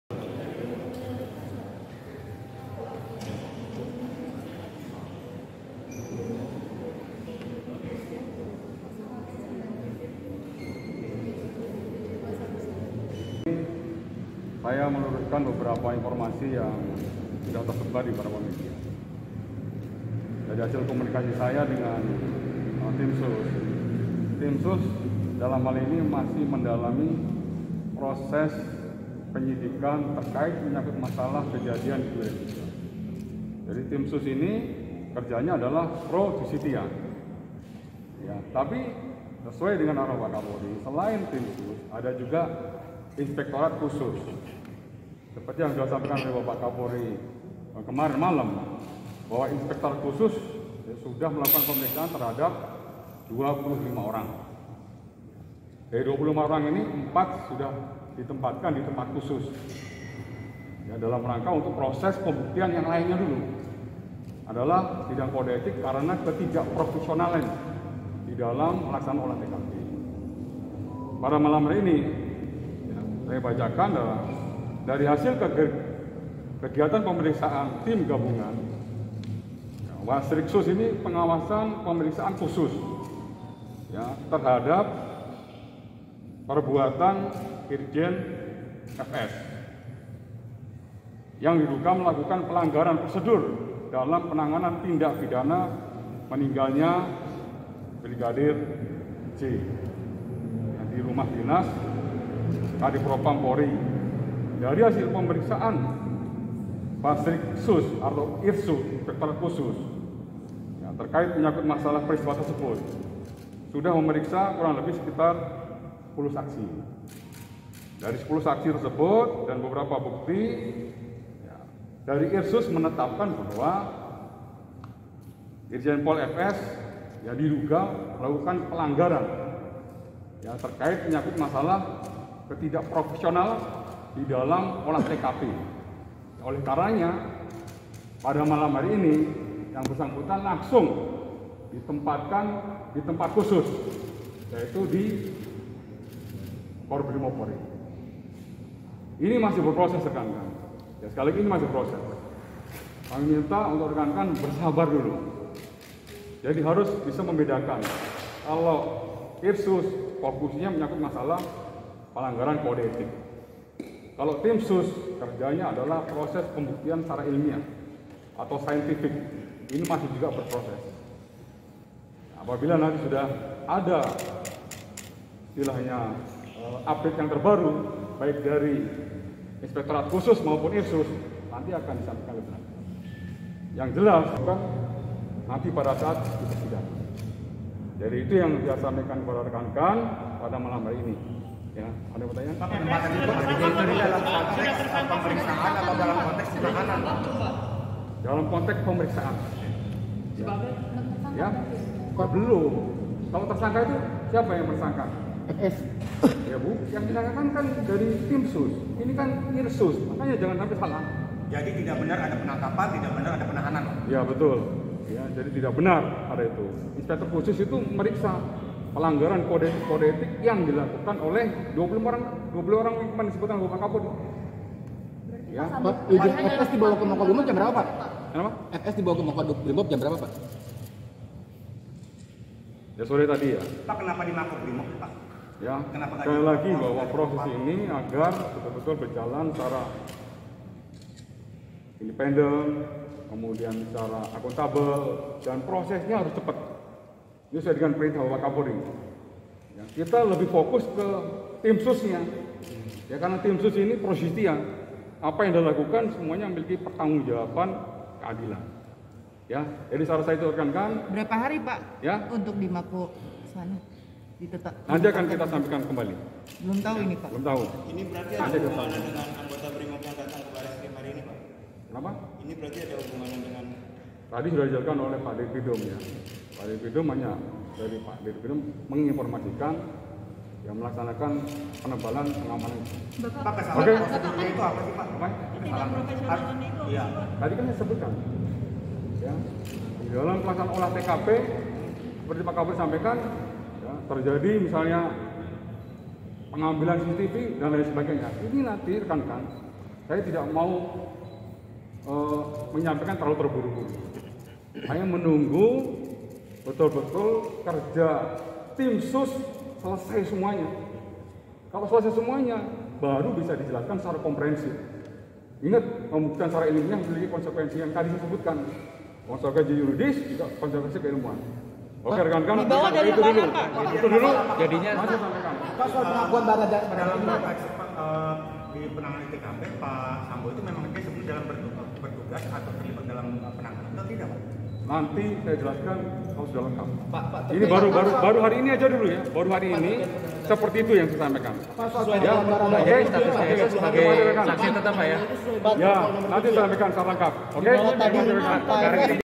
Mabes Polri Benarkan Irjen Ferdy Sambo Dibawa ke Mako Brimob
WARTAKOTALIVE.COM, KEBAYORAN BARU - Mabes Polri membenarkan Mantan Kepala Divisi Profesi dan Pengamanan (Kadiv Propam) Irjen Ferdy Sambo dibawa ke Mako Brimob, terkait kasus meninggalnya Brigadir J pada Sabtu (6/8/2022).